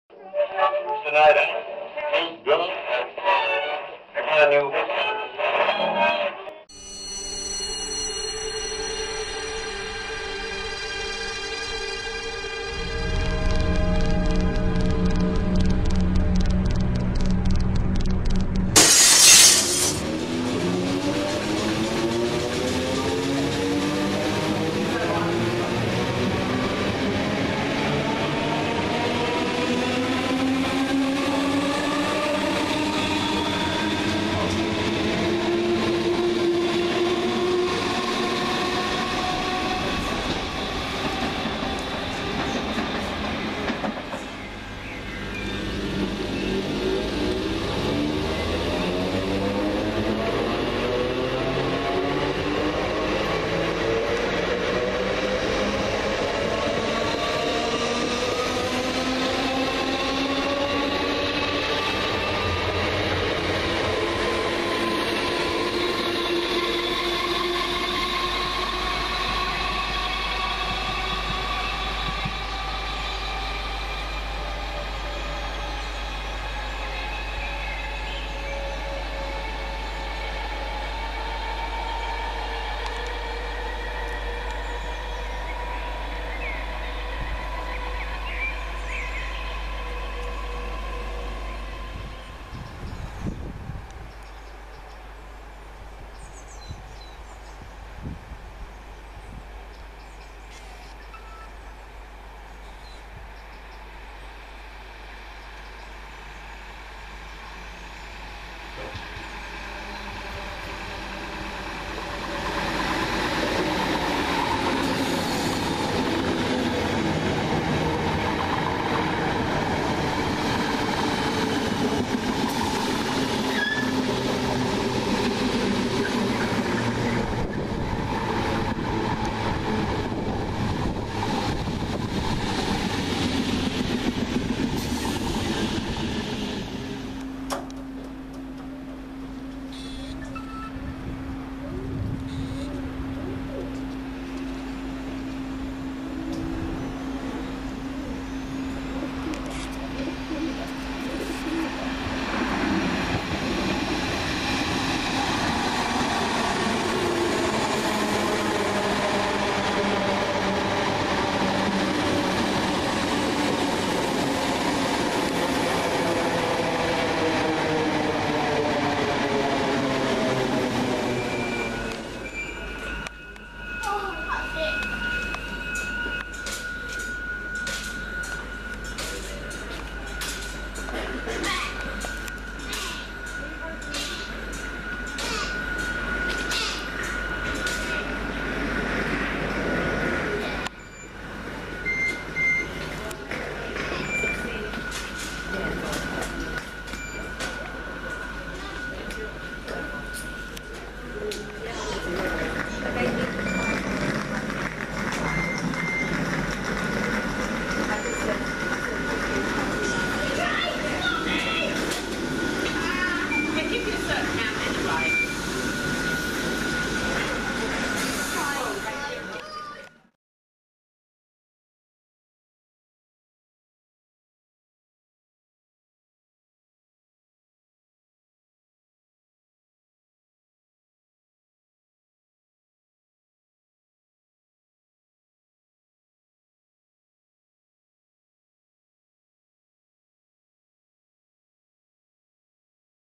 Season 7, Episode 300 - Highams Park
Our fourth station of the day brings us one station down from Chingford and brings us to a station with a country feel in Highams Park

In this video, we see a variety of London Overground stock heading for either Chingford or London Liverpool Street